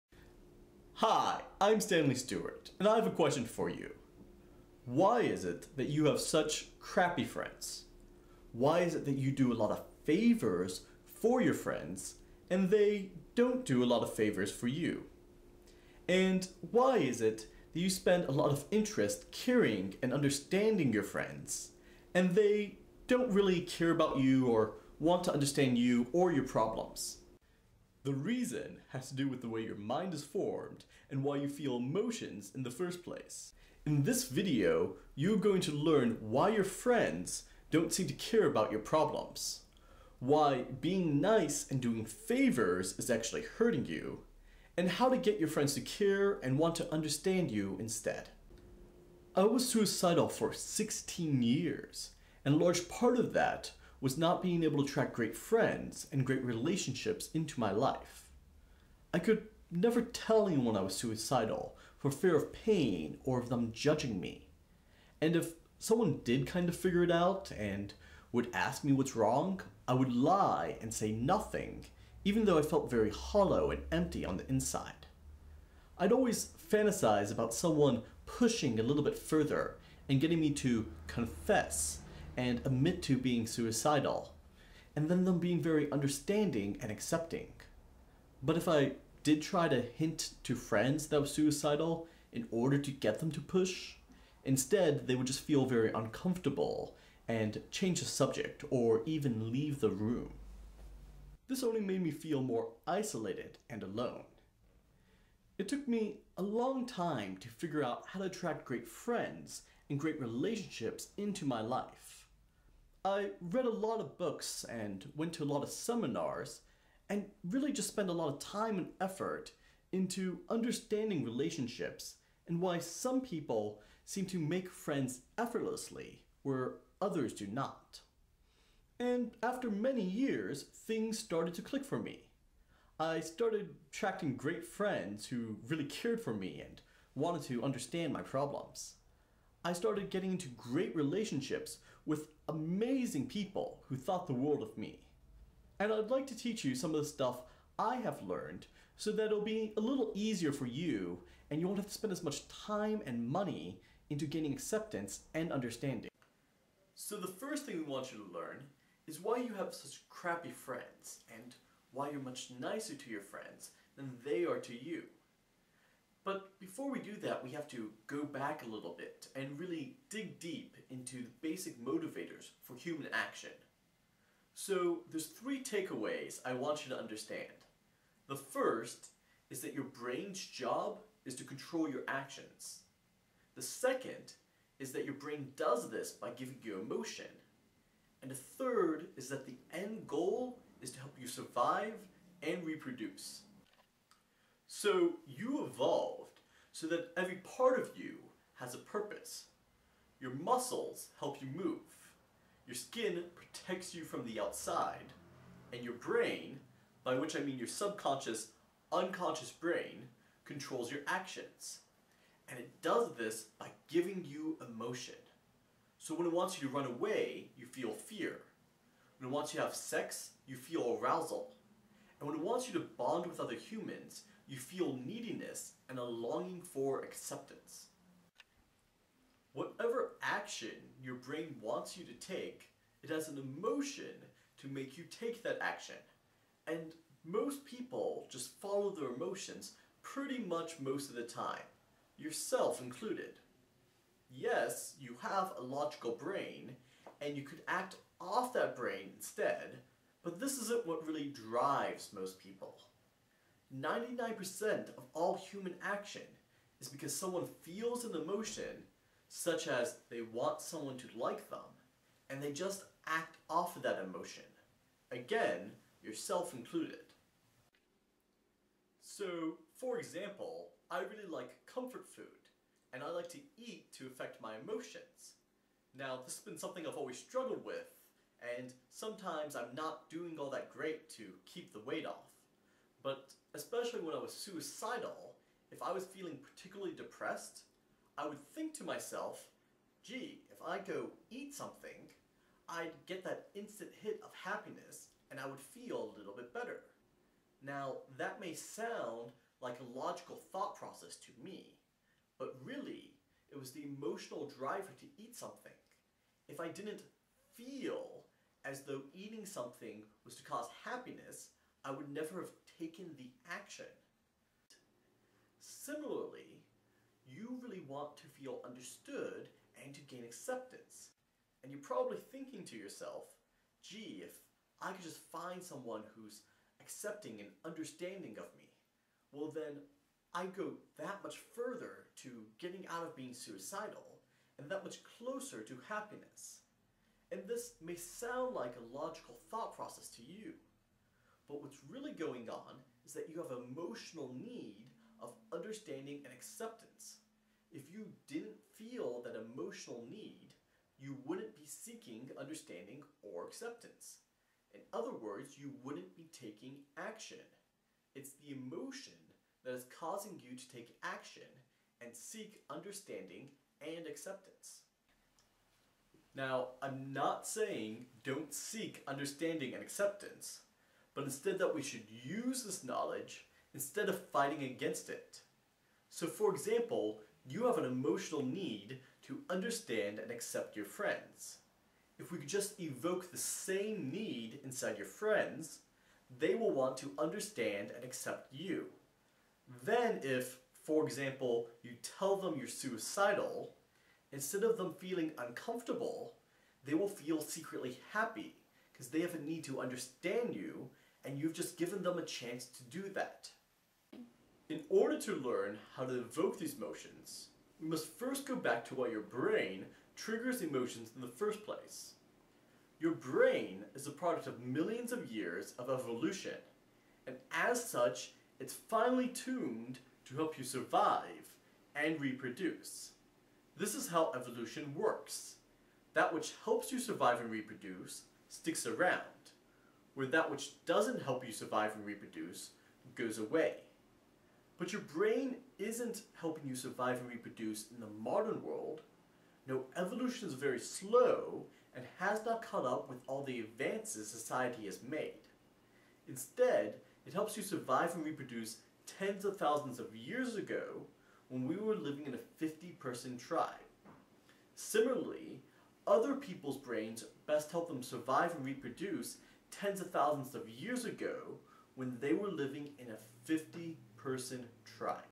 Why Your Friends Don't Care About You (& What To Do About It)- Part 1 of 4: Stop Suicidal Suffering
Most people who are suicidal and who would like to find someone who UNDERSTANDS them are making a very crucial mistake -- and they don't even realize it.

They are trying to get UNDERSTANDING by being extra NICE to people, doing favors for friends who WOULDN'T do favors back, and WAIT around for people to take an interest in them and just give CARING, UNDERSTANDING and ACCEPTANCE.

Even worse- on the off chance that someone does get around to asking what's wrong they usually LIE and say 'nothing', where they're really fantasizing that that person person pushes a little deeper and get them to CONFESS to their problems.

As you've probably discovered by now, this approach doesn't work. In fact, it can make the situation worse -
and cost you in other hidden ways that you didn't even consider.

So what DOES work?

If you'd like to find someone who UNDERSTANDS and CARES for you, and you'd like to do it quickly and easily, then you should watch this entire video.

And if you want to get more tips on how to IMMEDIATELY get out of being suicidal, go to: